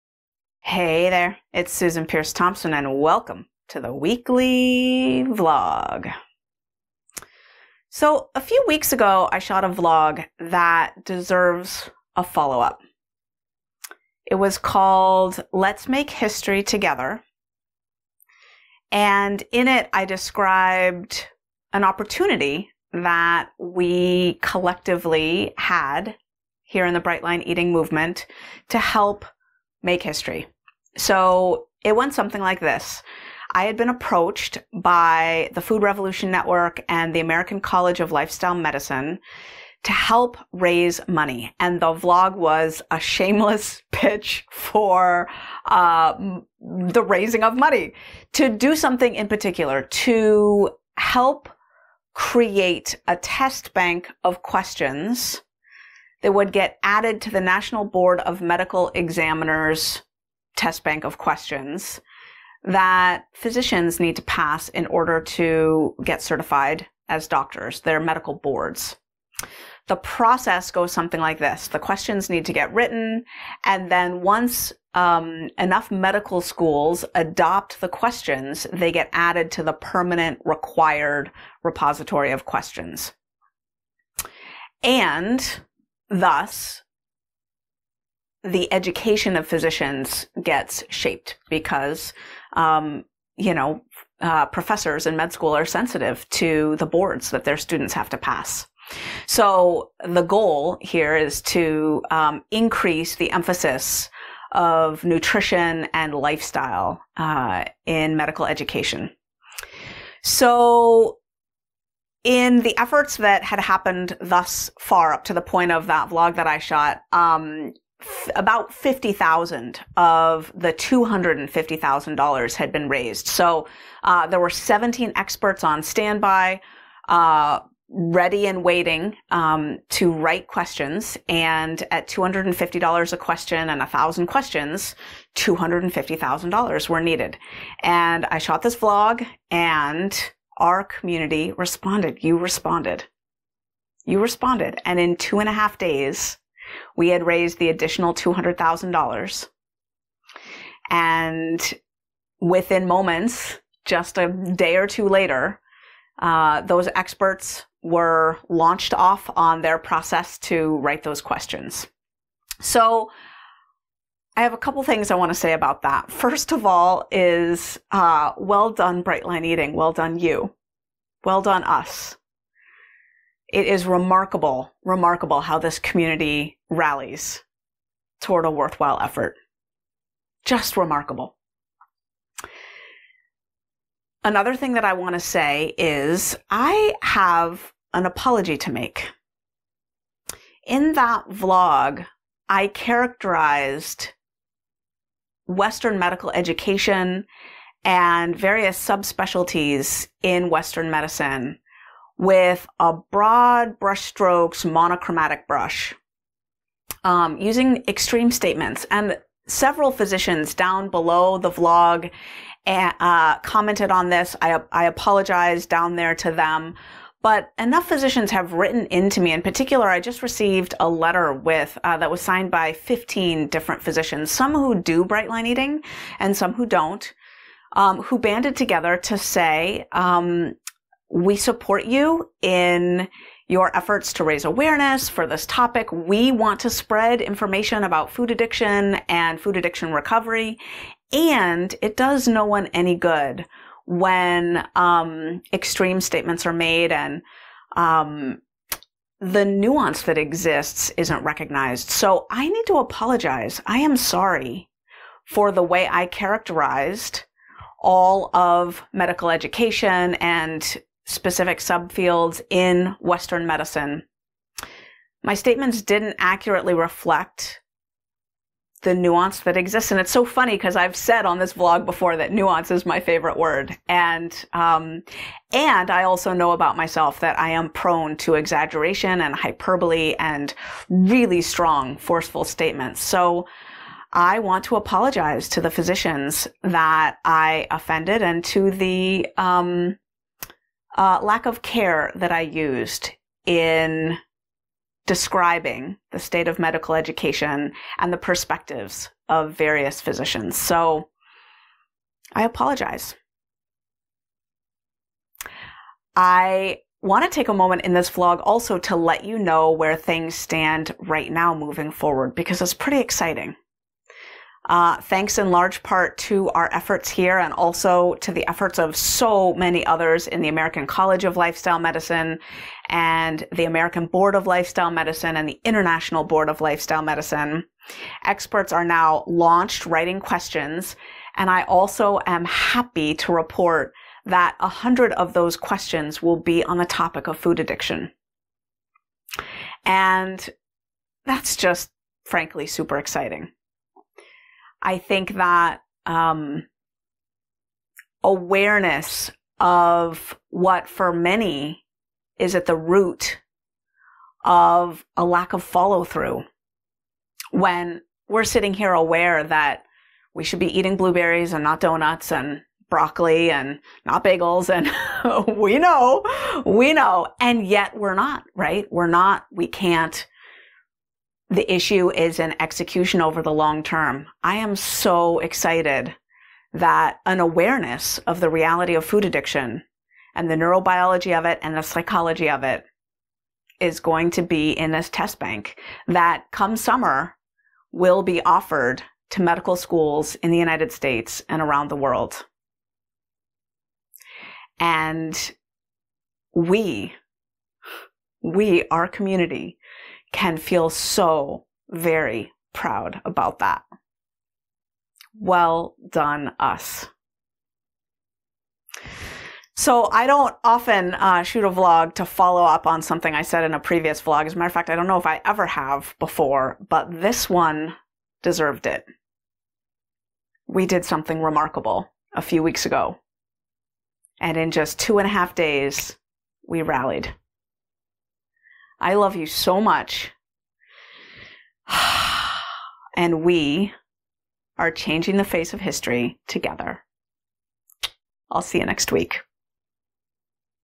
Lifestyle Medicine Follow-Up
A few weeks ago, I shot a vlog that really deserves a follow up. It was called "Let's Make History Together," and in it, I shared a momentous opportunity for the Bright Line Eating community. Watch this week’s vlog to hear what happened.